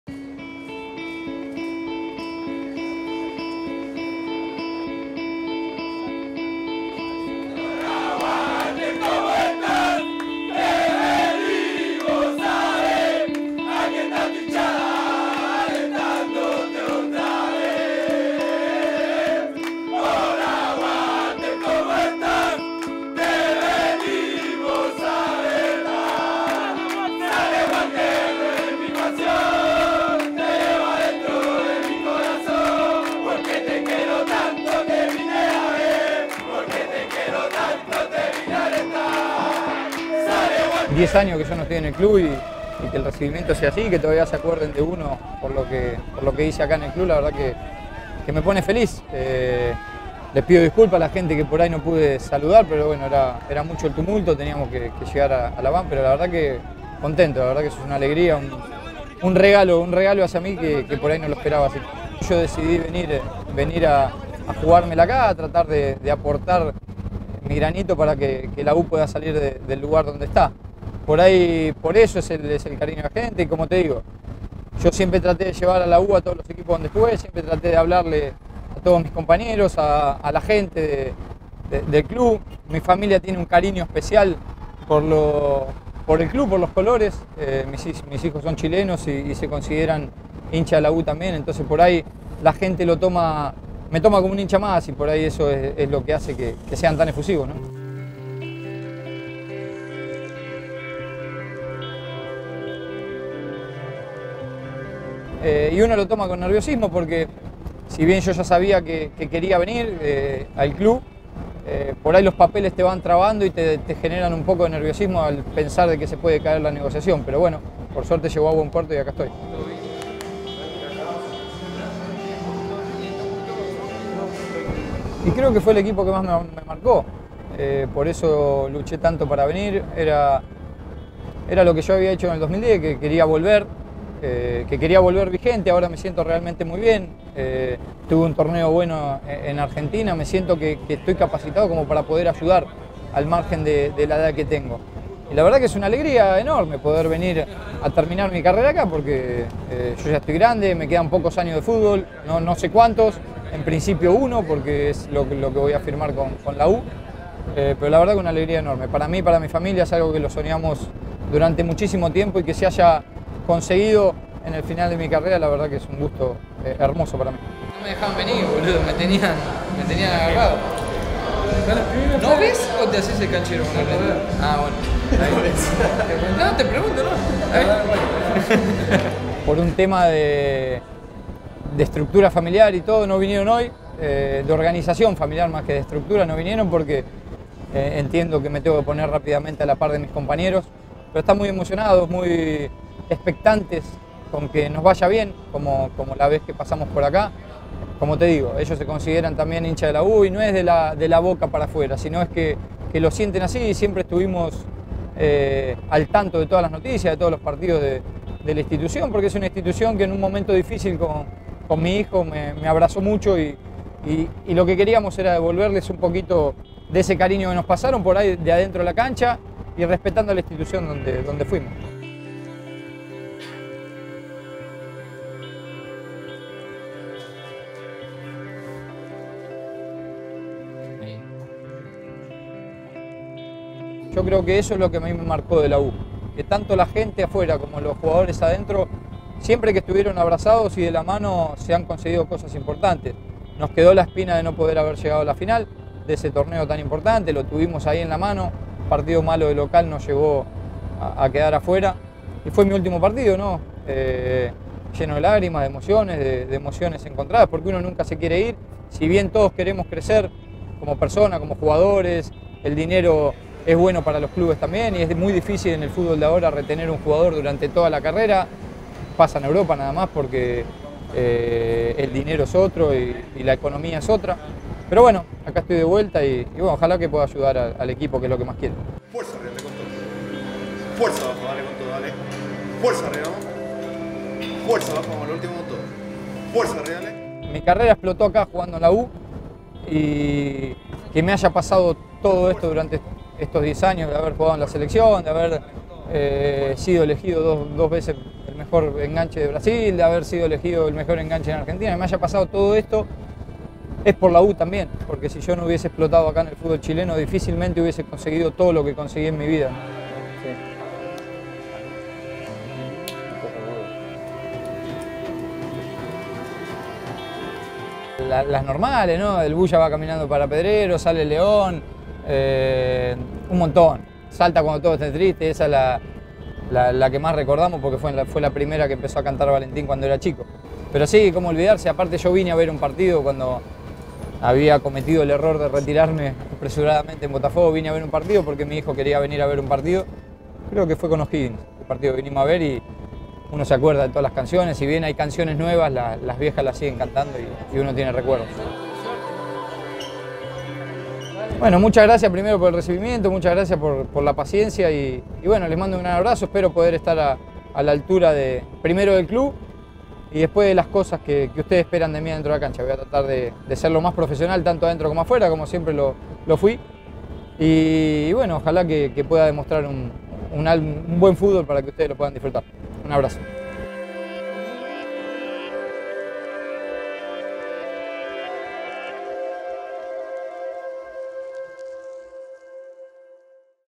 El retorno de Walter Montillo y su primer día en Chile
Su ansiado regreso ya está confirmado: Walter Damián Montillo es nuevamente jugador del “Romántico Viajero”. El volante argentino, campeón con la U en 2009, arribó este martes a Chile en medio de un efusivo recibimiento por parte de la hinchada azul. “Es una alegría enorme, lo soñamos con mi familia por mucho tiempo. Estoy de vuelta y espero ayudar al equipo”, comentó el jugador en sus primeras declaraciones durante una jornada que incluyó también la visita al CDA y posteriores exámenes médicos. Revisa cómo la “Ardilla” vivió su primer día. ¡Bienvenido a casa, Walter querido! ⠀